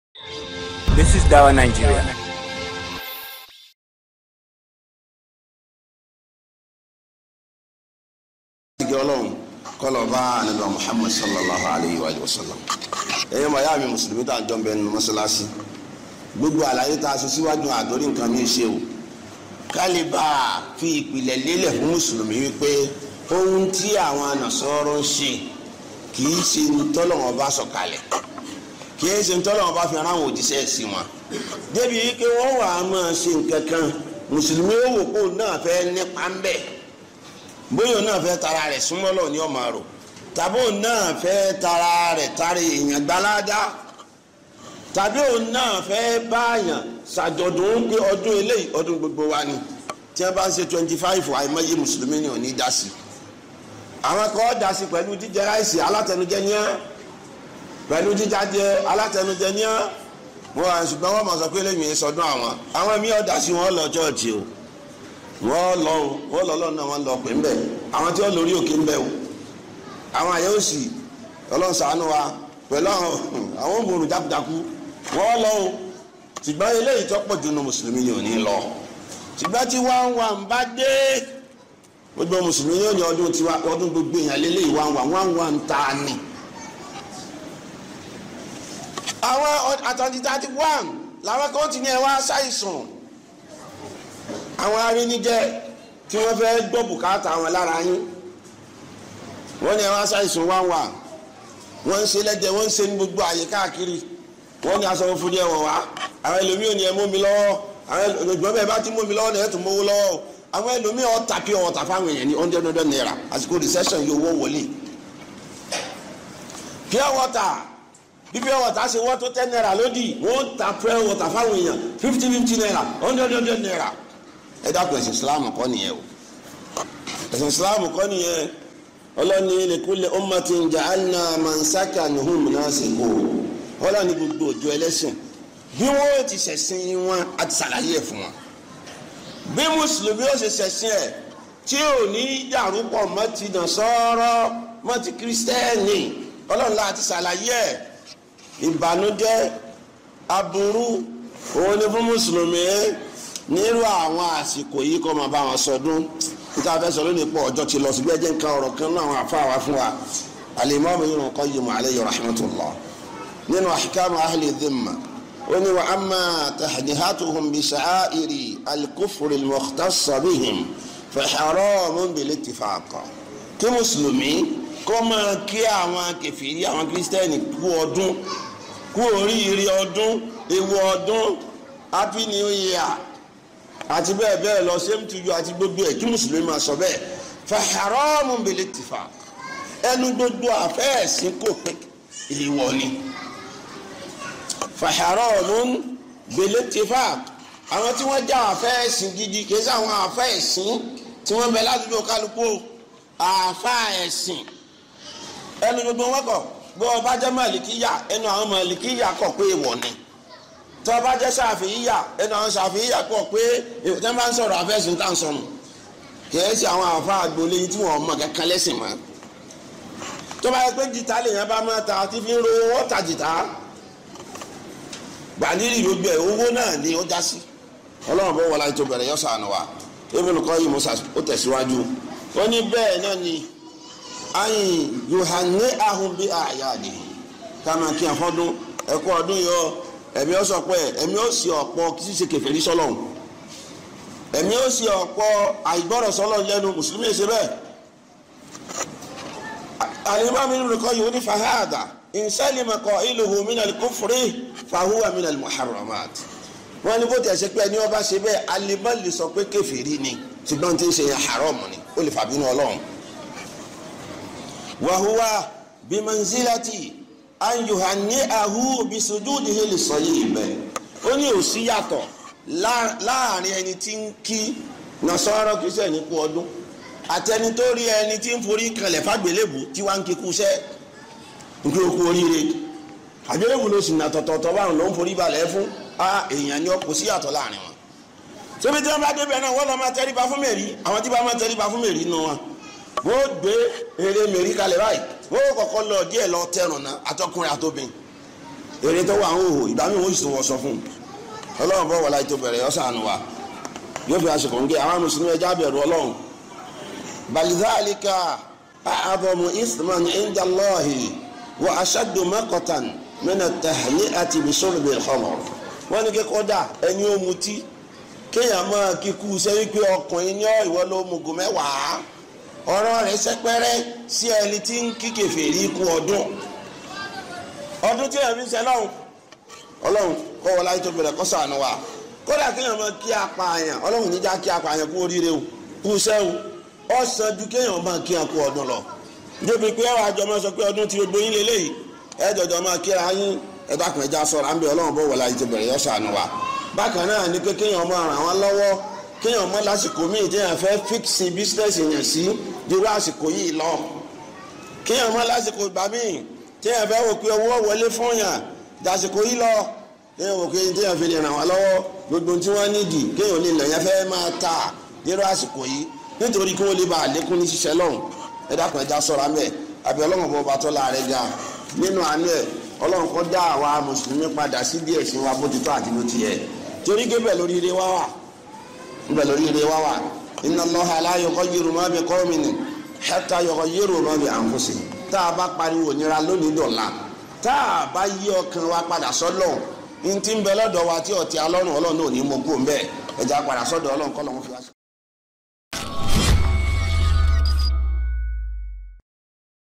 Enlightening Muslims on the Ills of Celebrating Christmas and New Year Ustadh HabeebuLlahi Al-Edewy
Enlightening Muslims on the Ills of Celebrating Christmas and New Year Ustadh HabeebuLlahi Al-Edewy (Ede)
There is no doubt that Christmas and New year celebrations are Haraam, because it is an imitation of the Kuffaar. It is well known that the Muslims do not have any festival apart from Eid al-Fitr and Eid al-Adha, and the weekly “Eid” which is Friday (Yawm al-Jumu‘ah).